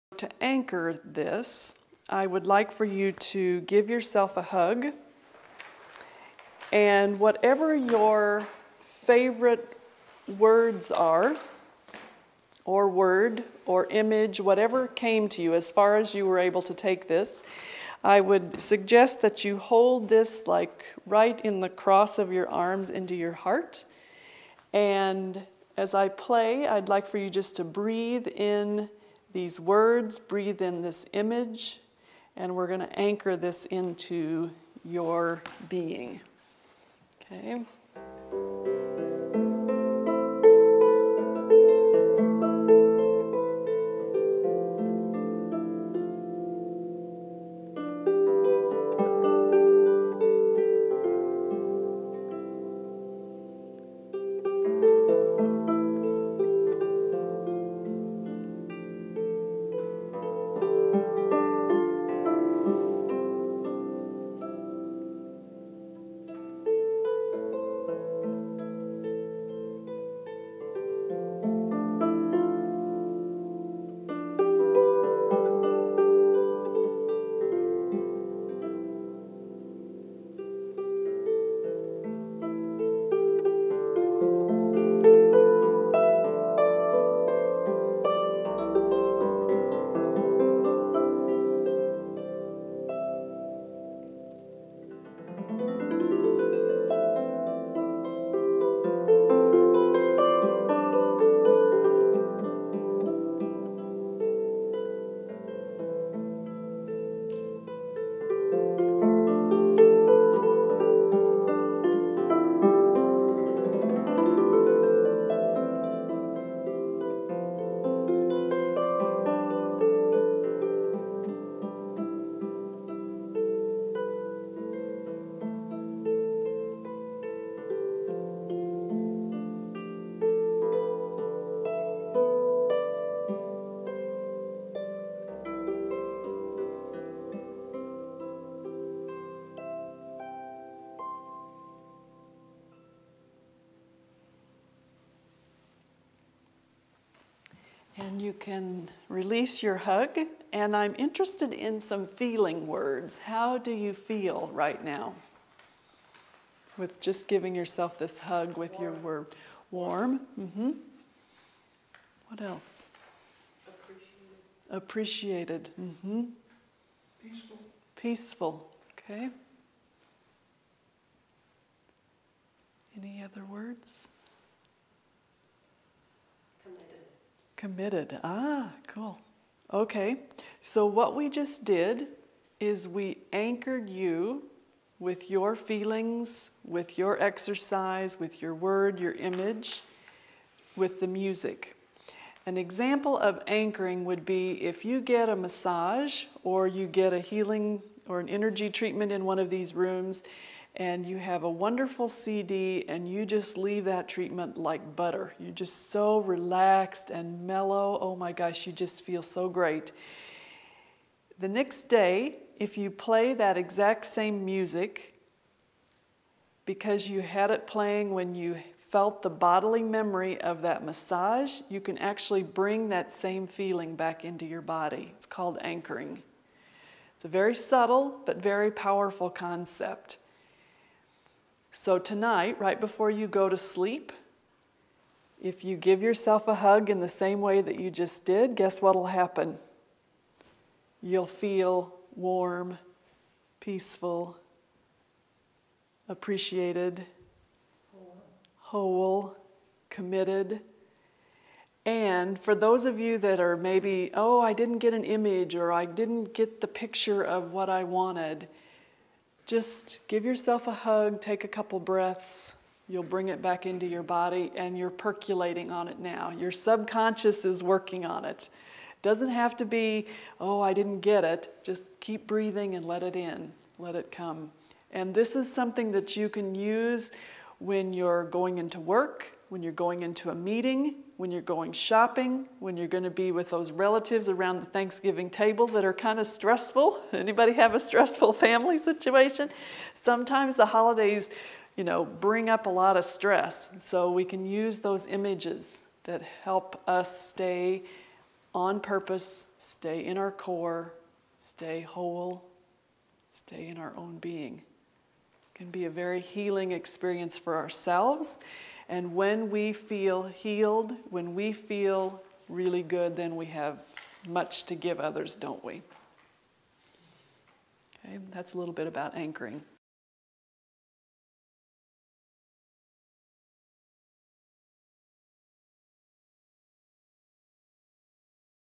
Healing Harp Concert/Presentation
Tami Briggs concertizes around the world. In this video, she shares wellness tips and her beautiful harp music.